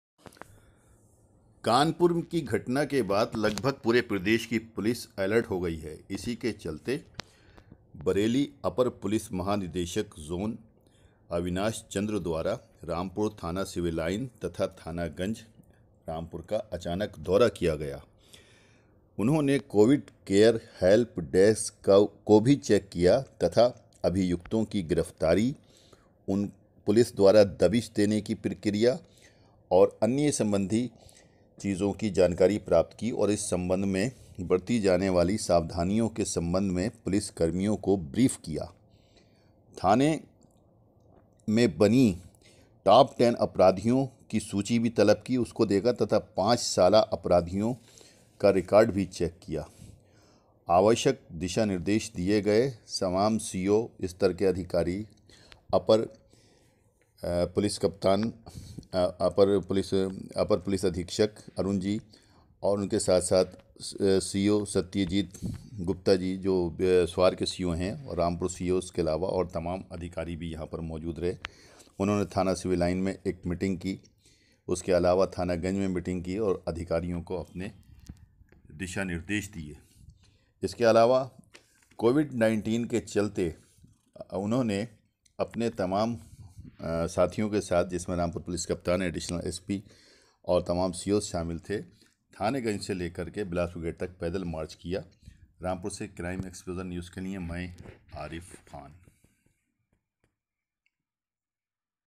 IG बरैली ज़ोन अविनाश चंद्र ने रामपुर दौरे में क्या कहा
देखिये ये दिलचस्प रिपोर्ट,,।